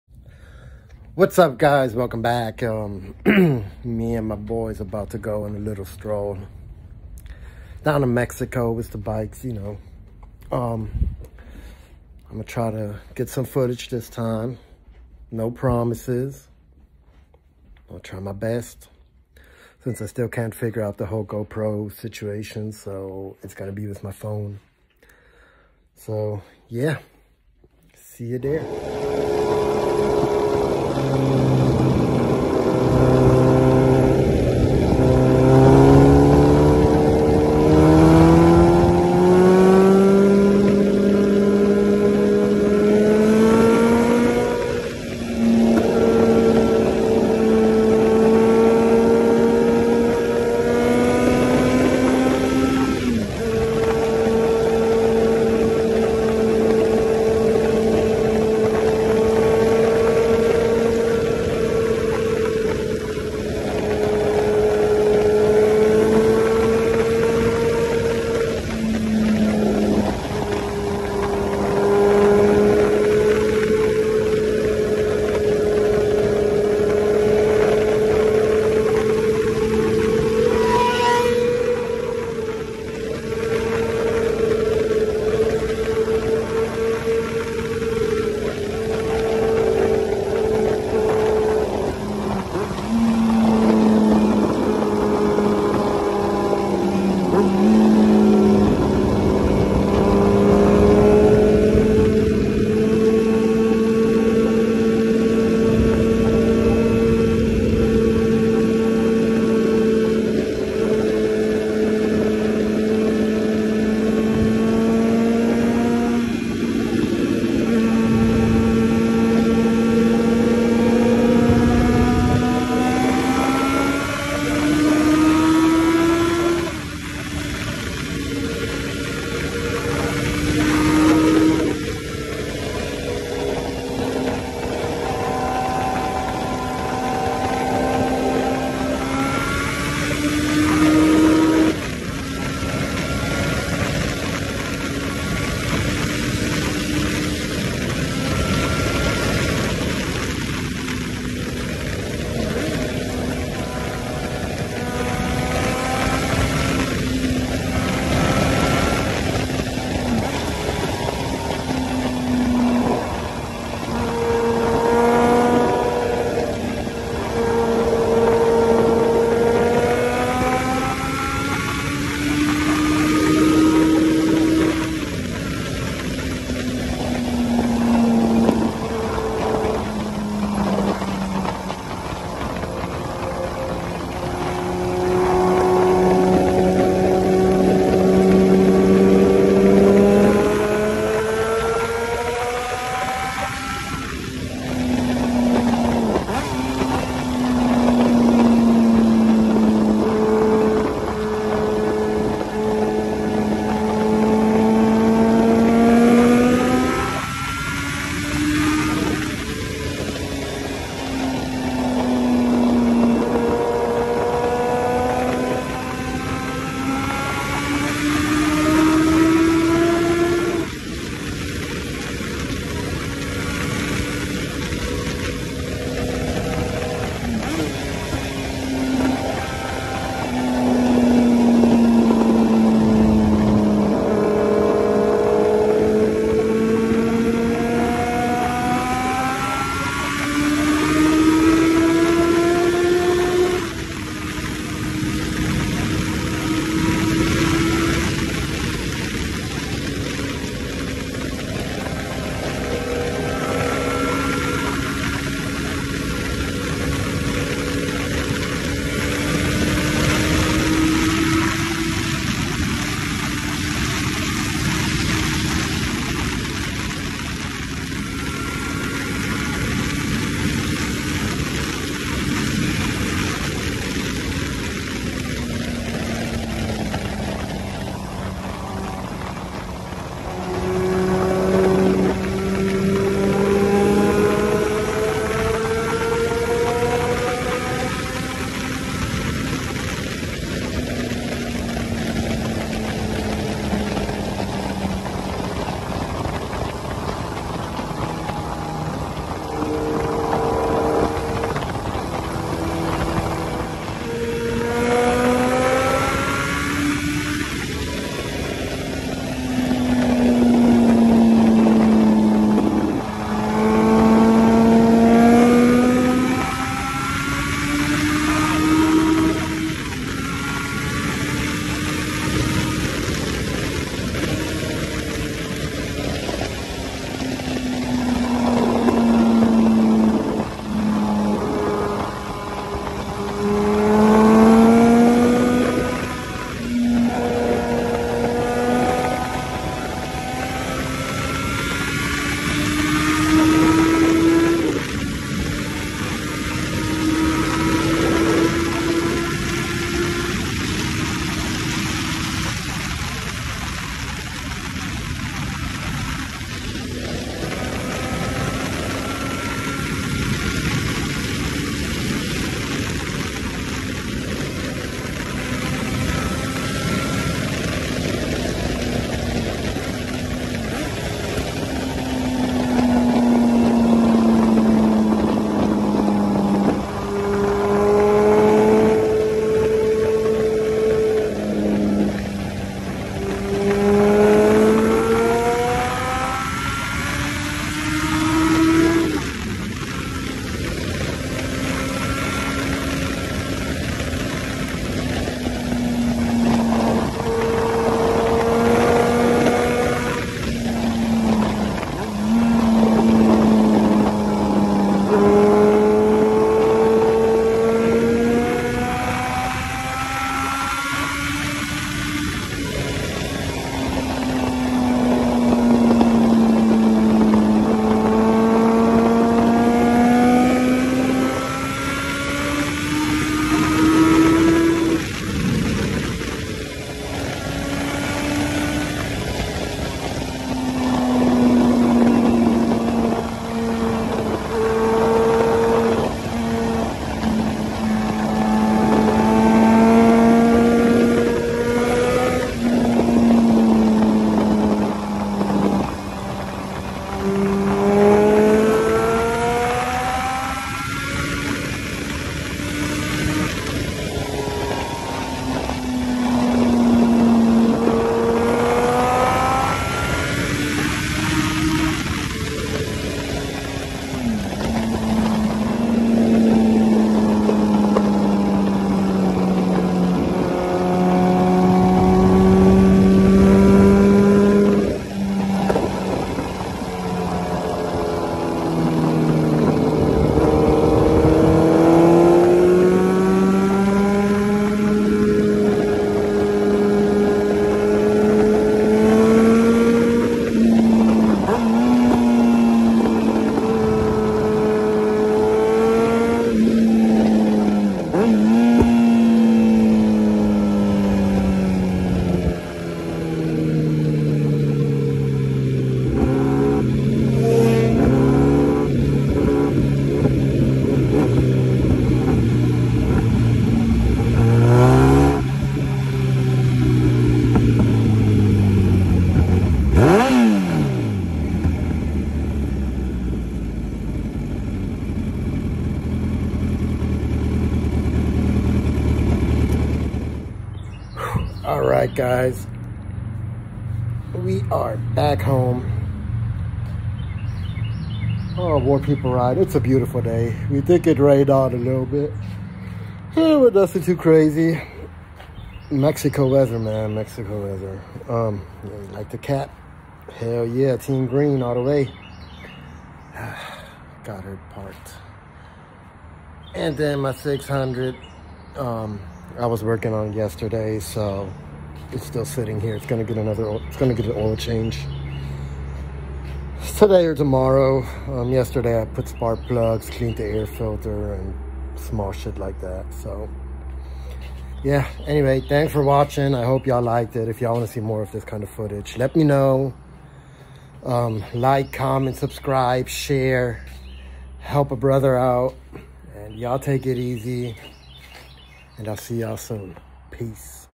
A group ride down in good ol’ Mexico!
Kenny G’s going away ride. That man hauls ass on his Harley sportster. Had a great time down in Mexico 🤙🏻